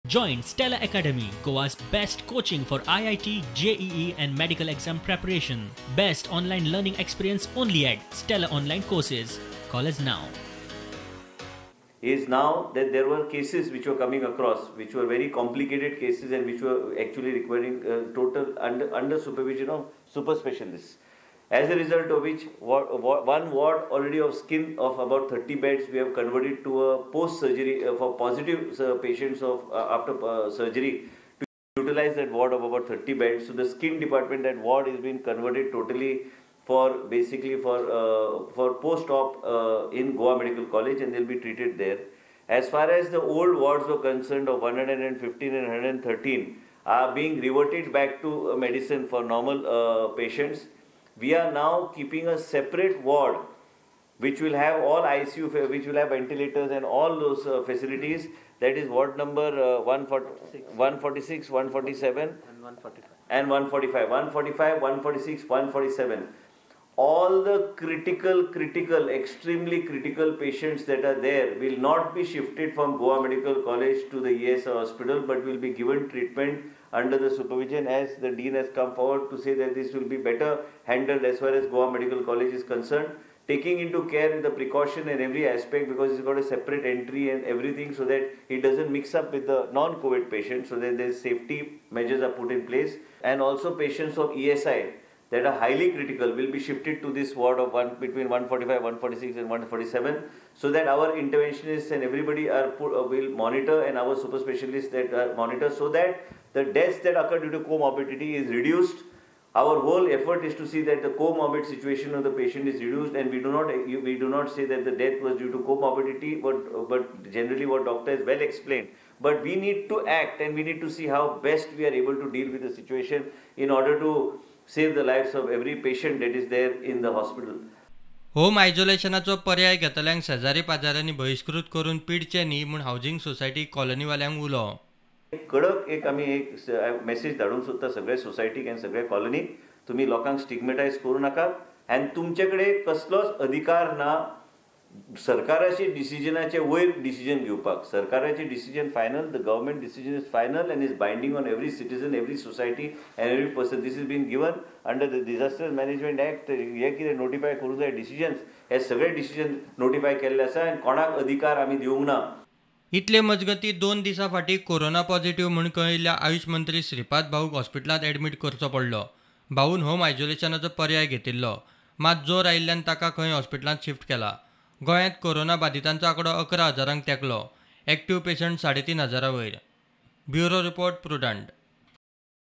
EXTREMELY CRITICAL COVID PATIENTS TO BE TREATED AT GMC: VISHWAJIT_Prudent Media Goa
Health Minister Vishwajit Rane says extremely critical patients will not be shifted to covid hospital and will be kept at wards 145, 146 & 147 of Goa Medical College and Hospital.

Critical patients at ESI hospital will also be shifted to GMC for treatment.

Rane says this decision has been taken to reduce co-morbid deaths.

Govt will take action against societies for causing stigmatization against covid patients who have opted for home isolation.

Health Secretary Nila Mohanan says 1st two patients who were plasma shots have tested covid negative.